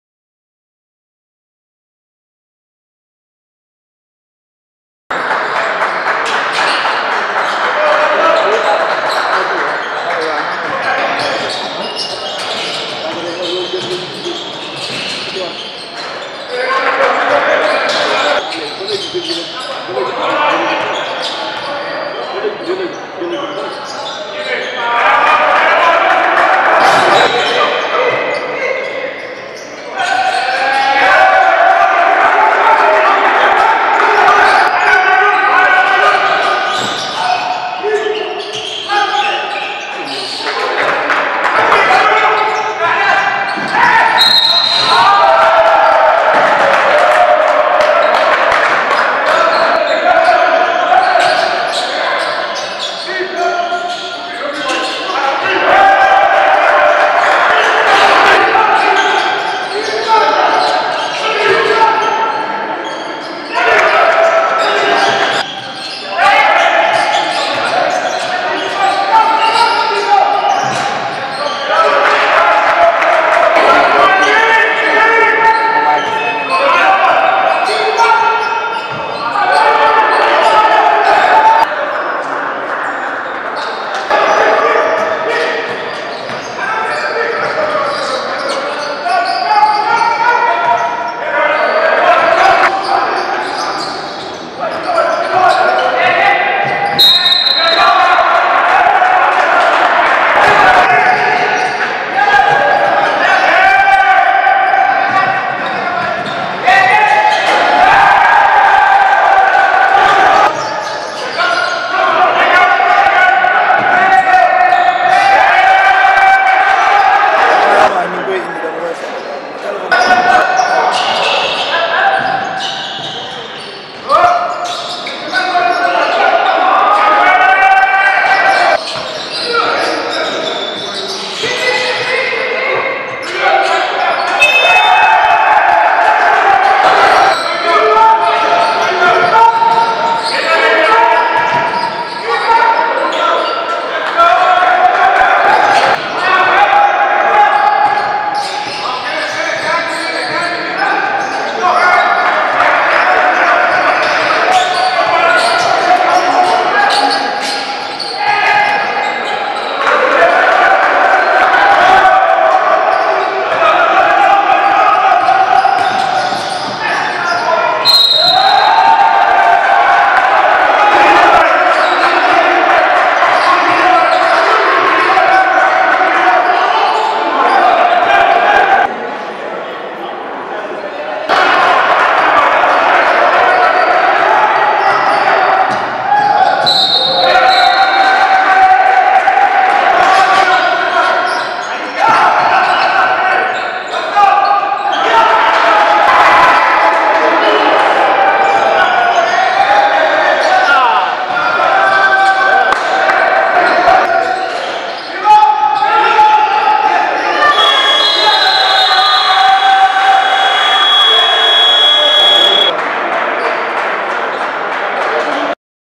MERMOZ vs UGB - 7eme Journée | D1 M. Sénégal | 08.03.2023 | BASKET221
Mermoz s'offre l'UGB et renoue avec la victoire